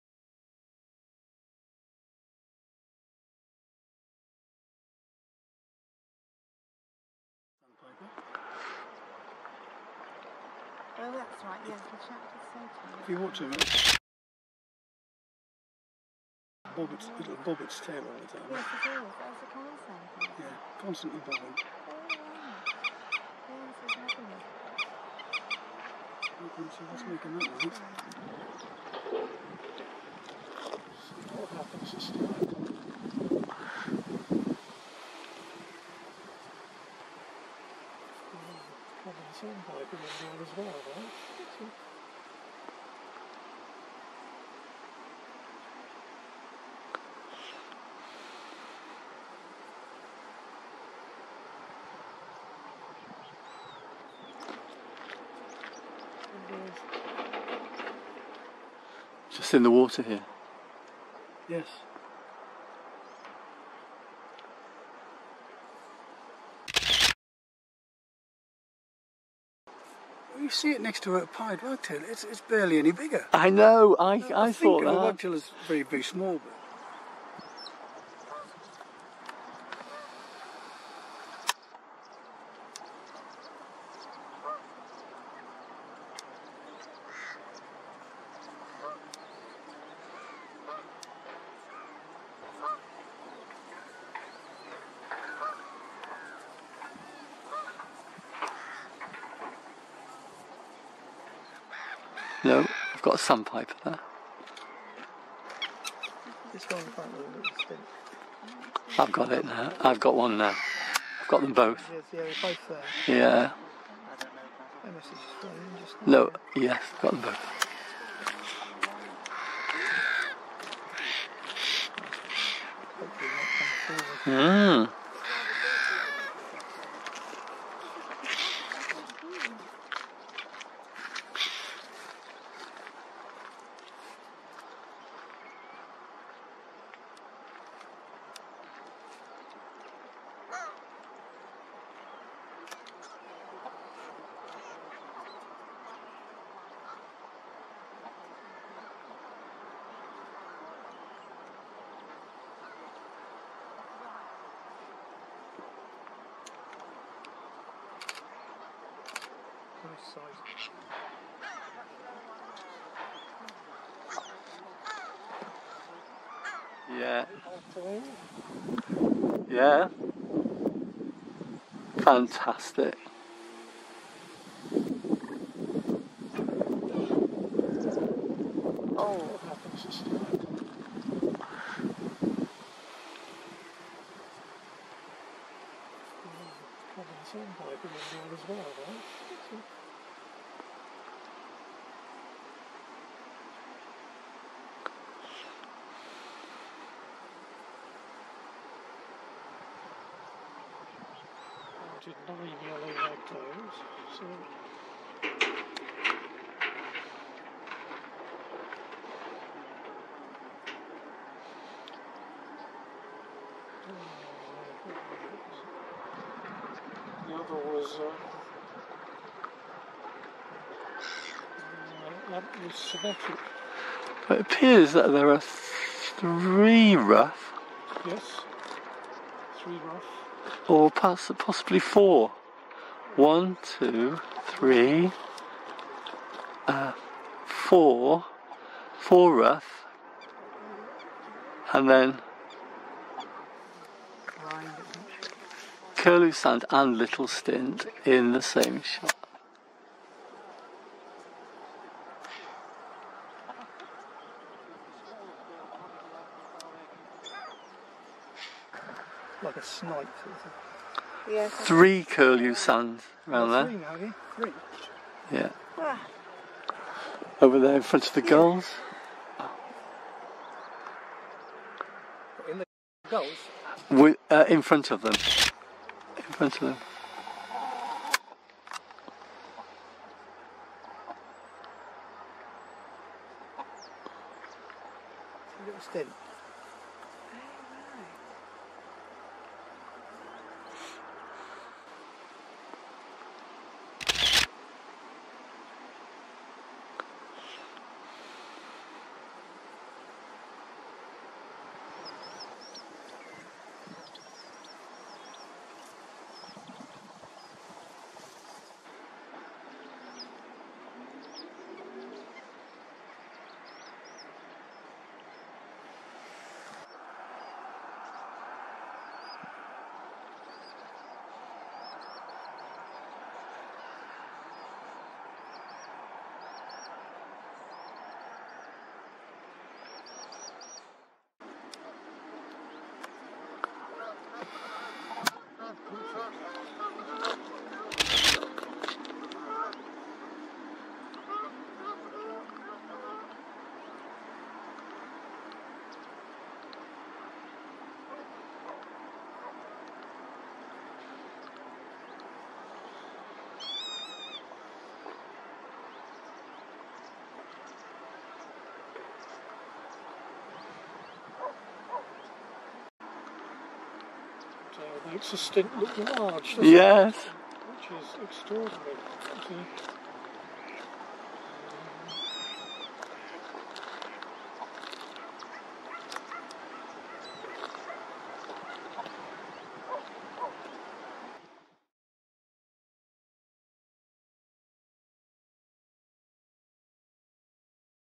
Lodmoor Birding #4 Samphire Island 4K
Other species seen on a Samphire-covered island in RSPB Lodmoor, including Common and Curlew Sandpipers, Dunlin, Little Stint, Lapwing, Ruff and Sandwich Tern.